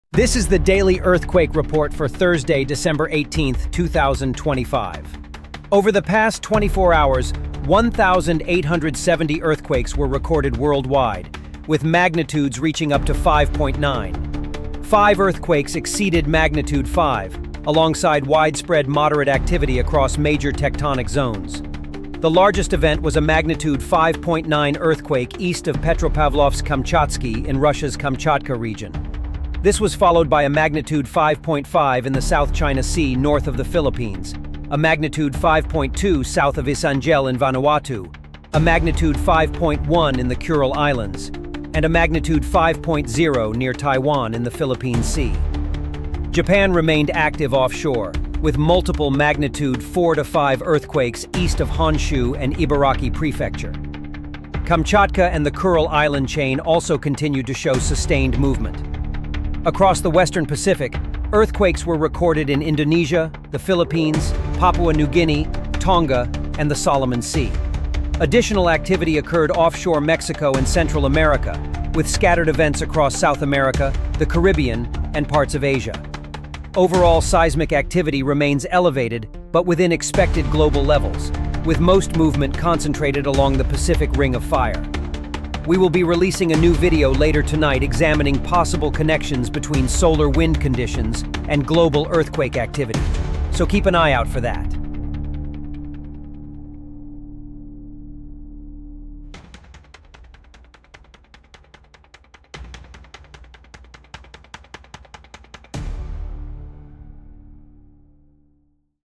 Daily Earthquake Report – Thursday, December 18
Over the past 24 hours, 1,870 earthquakes were recorded worldwide, with activity reaching up to magnitude 5.9. The strongest event occurred east of Kamchatka, Russia, and was felt across parts of the region.

Additional magnitude 5 and above earthquakes were recorded across the western Pacific, including the Kuril Islands, the South China Sea near the Philippines, Taiwan, and Vanuatu. Moderate activity continued across Japan’s offshore zones, along with ongoing movement across Southeast Asia, Central America, and the South Pacific.

Global seismic activity remains elevated but stable, with no immediate escalation patterns identified at this time. Monitoring continues across all active regions.

More updates if anything changes.